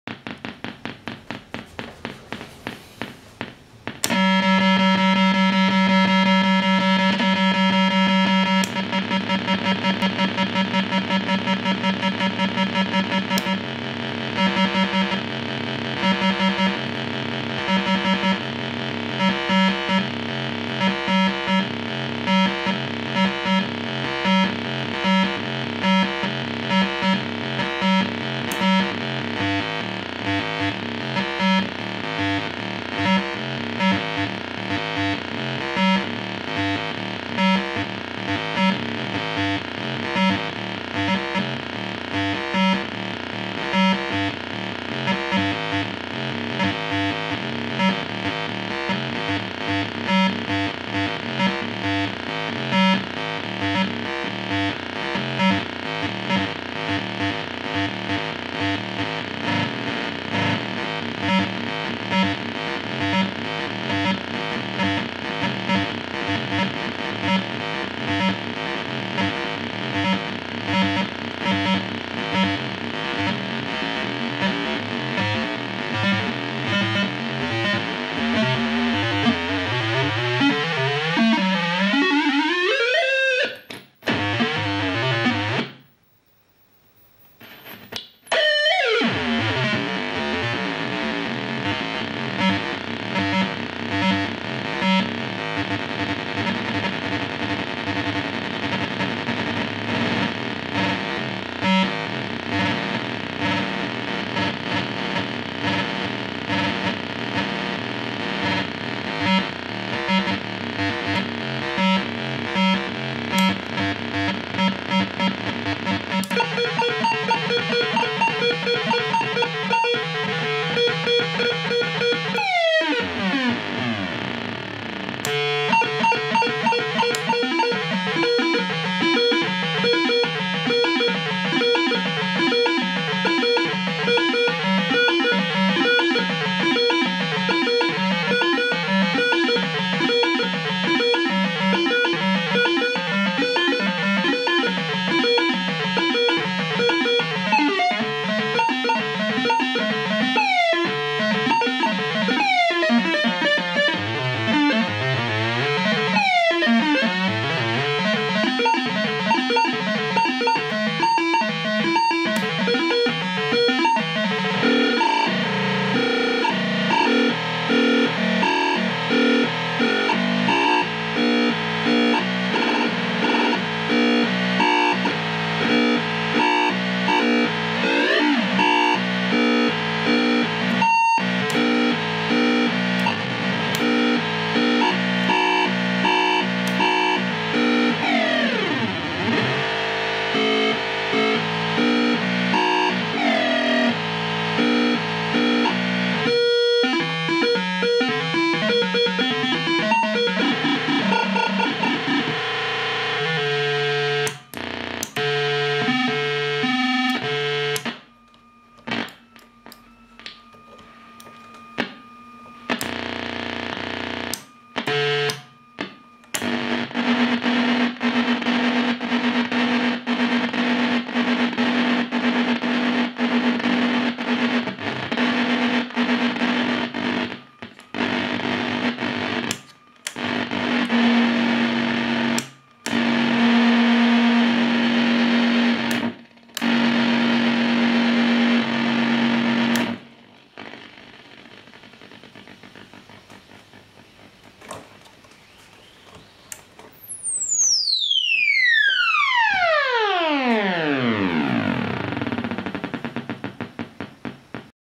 arp-error
.new toy for the music lab. based on 4051. pcb available. cheers.